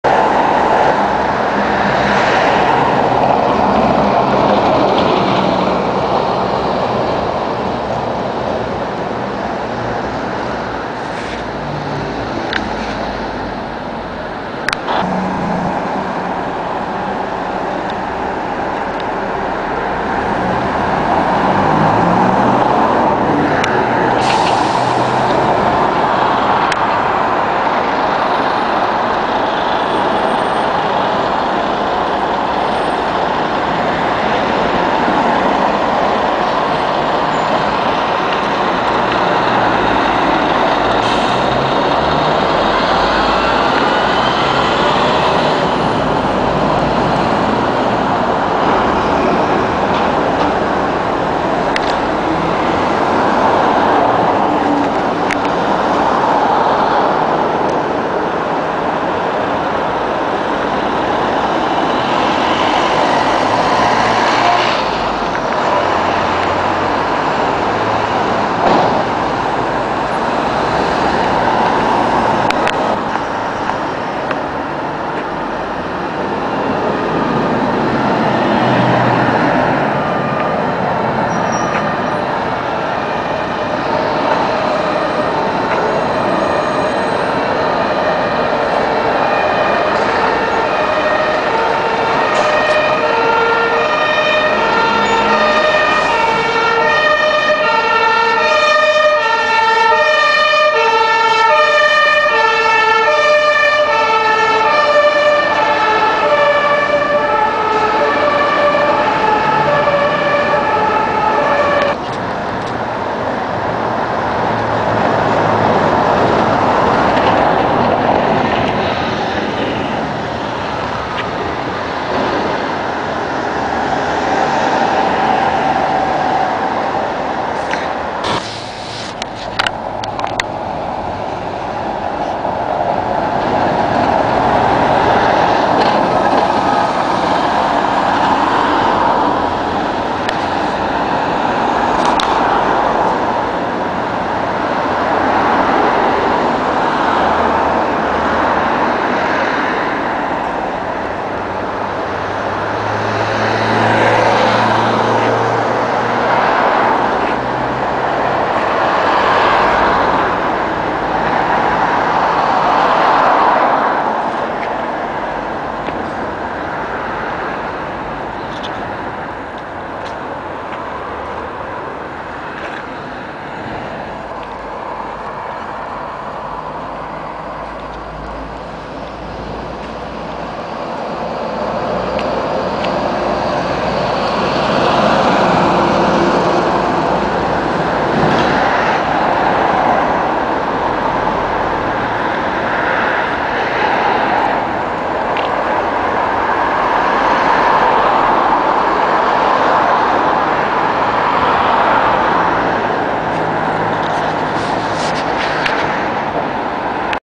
Bus Rapid Transit style bus lanes in Brussels
Effective bus lanes in the median of wide street in Brussels. You can see how the lanes are physiacally protected from other traffic. At 1:30 you can see a lory waiting for a bus instead of overetaking it.
Where: Avenue Pont de Luttre